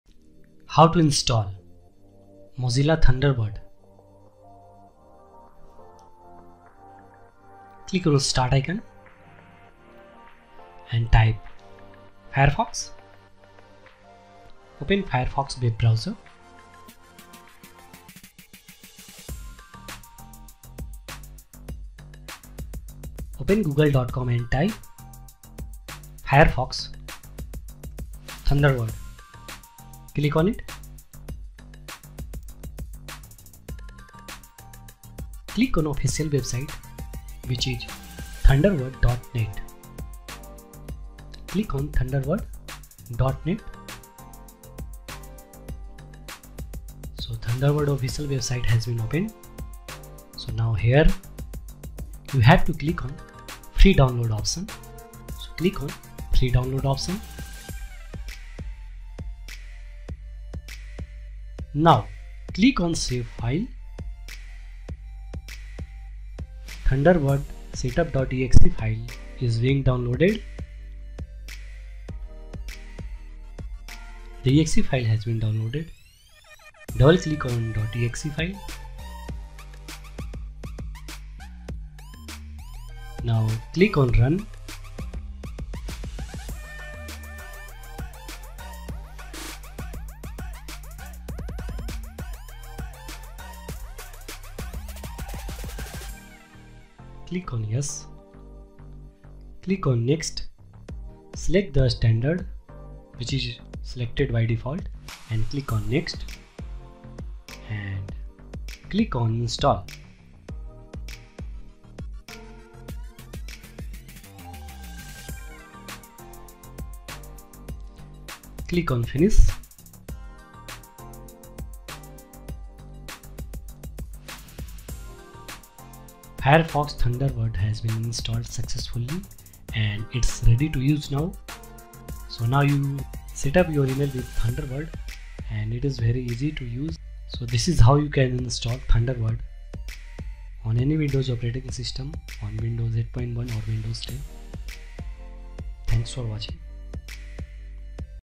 How to download and install Mozilla Thunderbird email application on Windows 10, 8.1, 7 OS / SE
How to download and install Mozilla Thunderbird email application on Windows 10, 8.1, 7. Today, i am going to talk about how to install Mozilla Thunderbird email software on your windows operating system. The Thunderbird email application is far better and easy to use than Microsoft Outlook.

So, follow the steps...

How to install Mozilla Thunderbird
Click on start icon
Search Firefox and open it
Now Thunderbird official website has been opened
Click on save file
Thunderbird setup file is being downloaded
Click and open .exe file
Click on Run
Click on next , next & install
Click on finish
Thunderbird has been installed successfully and ready to use

Thanks.
Smart Enough YouTube Channel